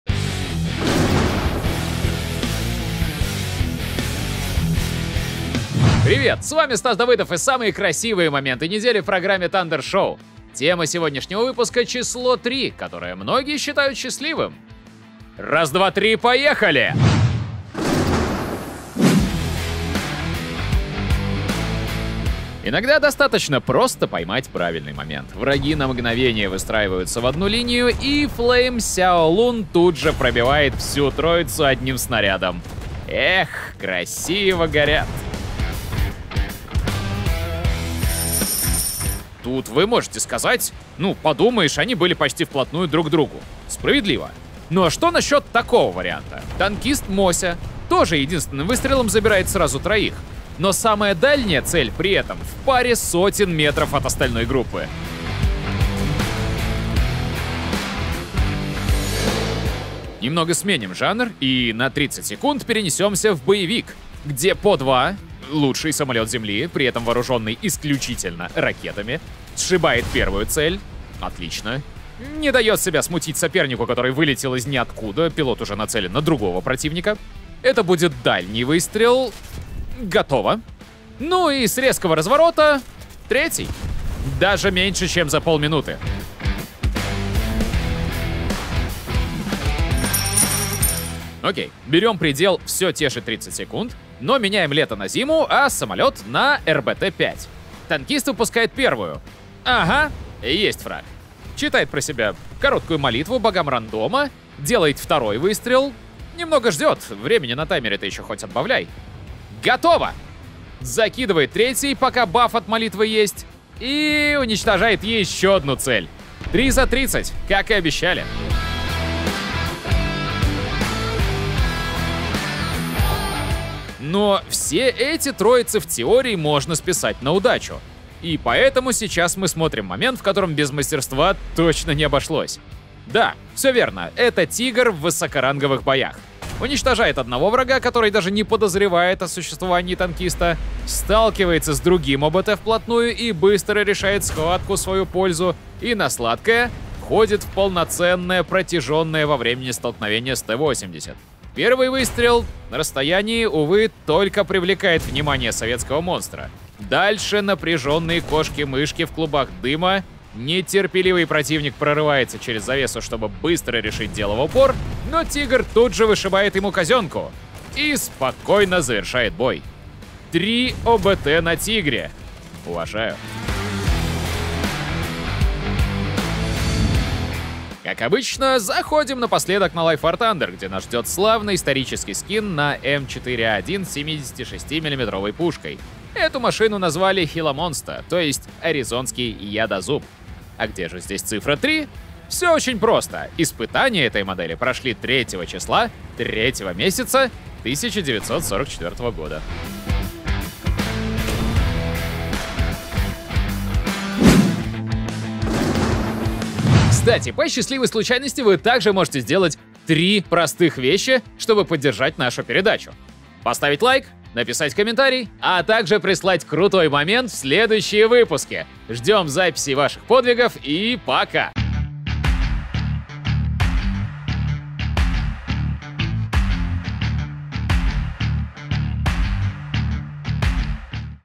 Thunder Show: ТИГРОПАРД
Еженедельное шоу, которое мы делаем вместе с вами!

Тема этого выпуска — число “три”, которое часто считают счастливым.